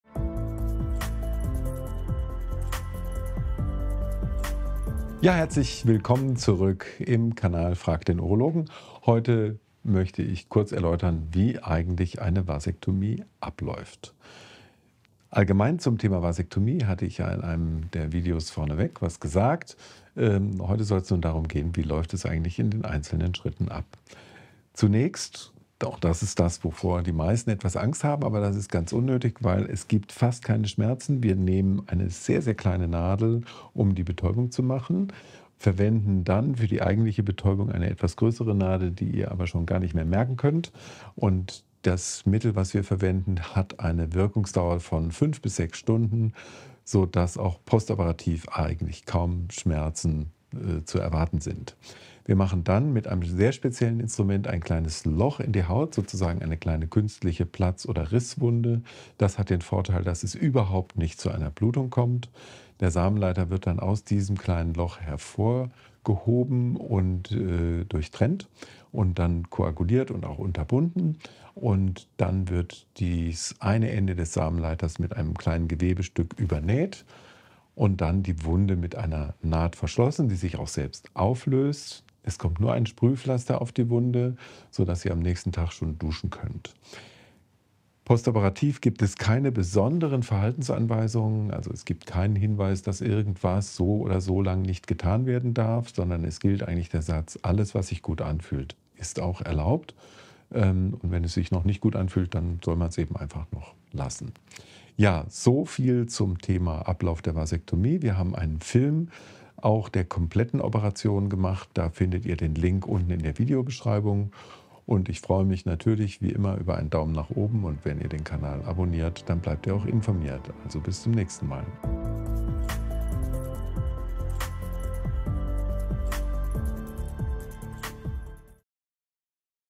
Der Ablauf einer Vasektomie Operation (mit Bildern der OP) | Frag den Urologen
Deine Männerfragen: anonym gestellt – kompetent beantwortet!
Der Ablauf einer Vasektomie-Operation (mit Bildern der OP).